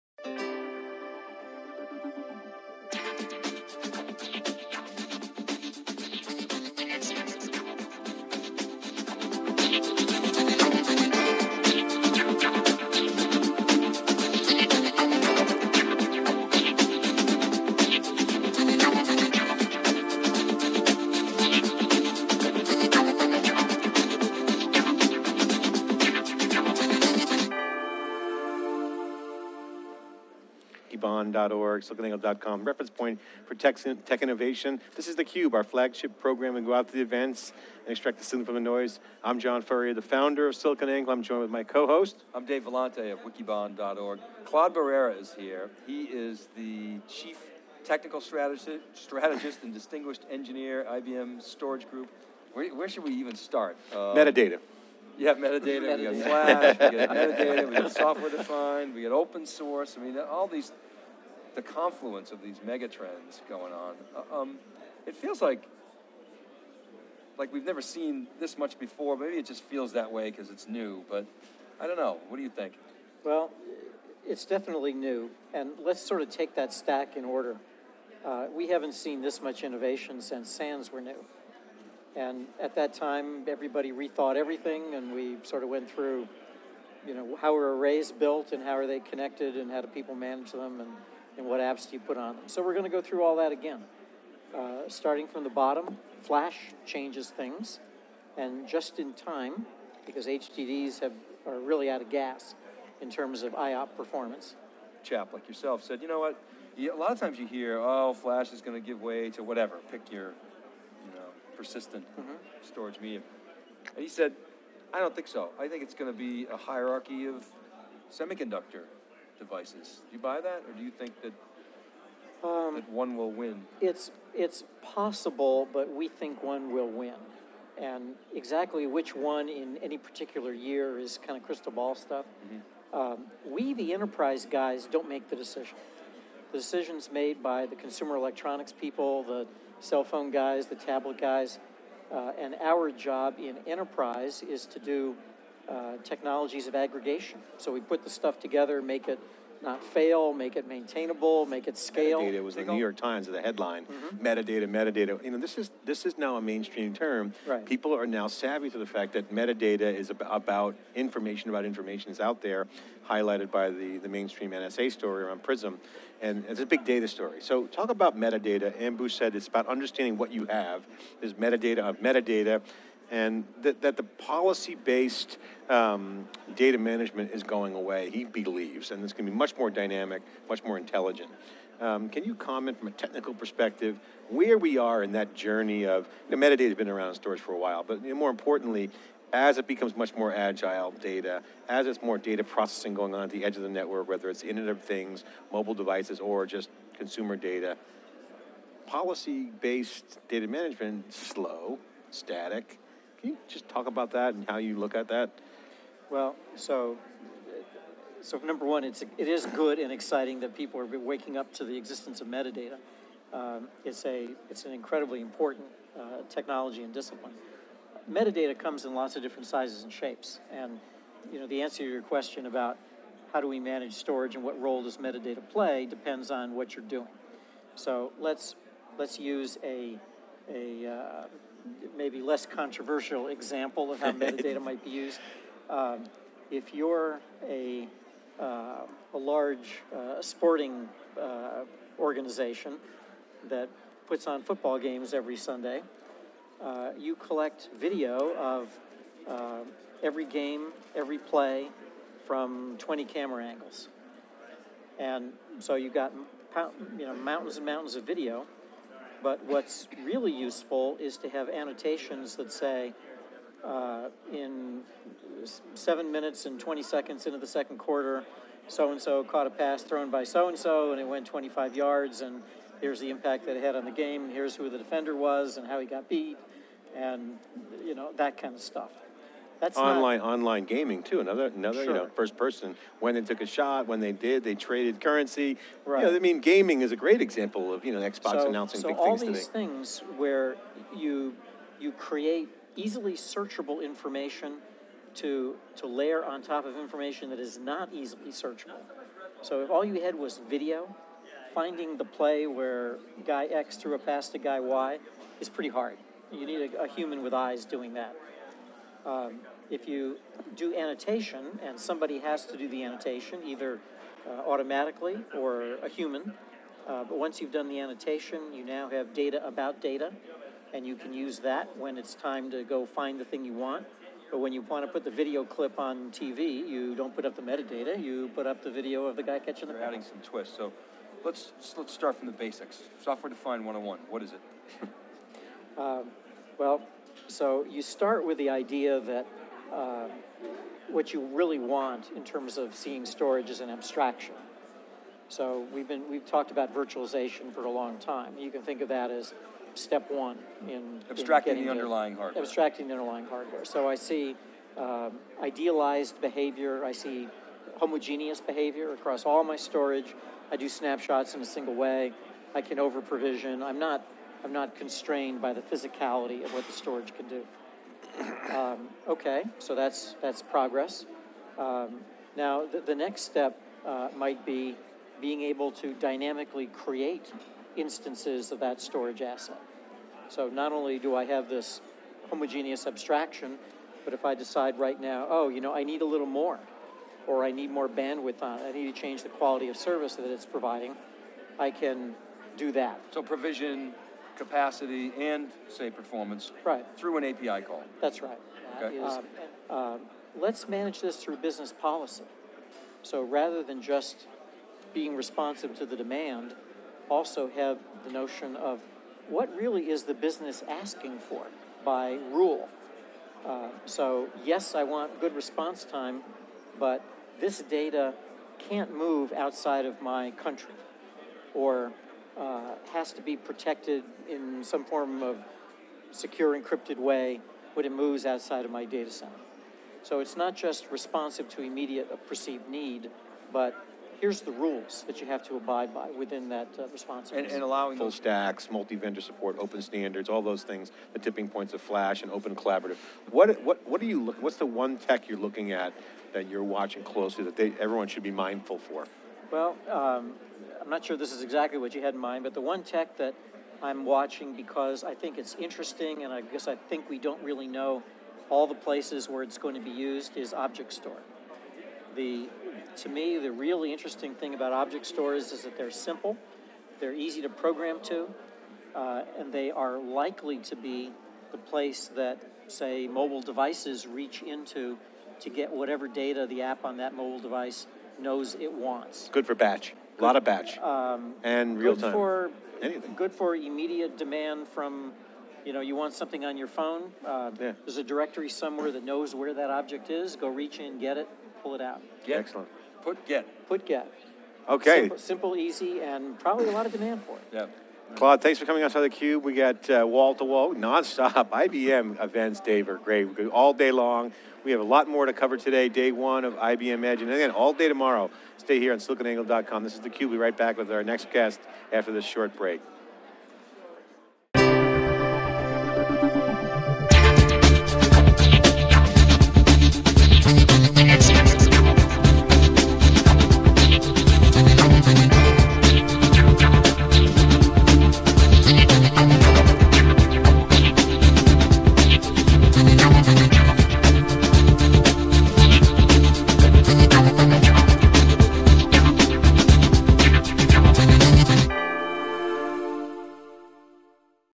Clod Barrera, IBM | IBM Edge 2013 - Highlights
Clod Barrera, IBM, at IBM Edge 2013 with John Furrier and Dave Vellante

In their onogoing coverage of IBM Edge 2013, John Furrier and Dave Vellante and John Furrier spoke with IBM Chief Technical Strategist, Storage, Clod Barrera. The three discussed flash, metadata (particularly in light of NSA concerns), and what Barrera believes is an important tech trend.

Barrera explains that when it comes to future applications of Flash, enterprise technologists are not the decision makers. Rather, consumer electronics technologists (the folks behind tablets and smartphones, for example) that decide the relevance of Flash.

Turning to topical news, Furrier asks for Barrera's thoughts on meta data and policy based data management in light of the NSA revelations. Barrera notes that it is good and exciting that people are waking up to the existence of metadata. He also offers a basic overview of metadata and the fuzzy areas that make it unclear and to what extent privacy is violated through government data gathering. Barrera uses the metaphor of football game coverage. If a large sporting organization hosts football games every Sunday, it can collect video of every game, every play, from every camera angle. But, what is, perhaps more useful than the coverage itself, is the aiblity to annotate it to describe what happened with every blitz and tackle, for example. These annotations of video, constitute data about data, or metadata. And, in some instance, Barrera notes, "metadata can be significantly bigger in size than actual data."

Policy concerning access to citizen data can be a complicated issue. In some instances, one aspect of our individual data may be private, while another may be less private. For example, the content of our phone conversations should be private, but data of who we call is more open as phone companies have access to this information.

Discussing the importance of software defined technology, Barrera explains, "I see homogeneous behavior across all my storage, I'm not constrained by the physicality of what the storage can do." He suggests the next step is to create more bandwidth as well as provision capacity and performance through an API call.

Furrier concludes by asking Barrera what is "the one tech" he's focusing ont that everyone should be looking towards. Barrera cites ObjectStore  as simple, easy to program and good for immediate demand as in when you want something on your phone, it can reach in and cover it quickly.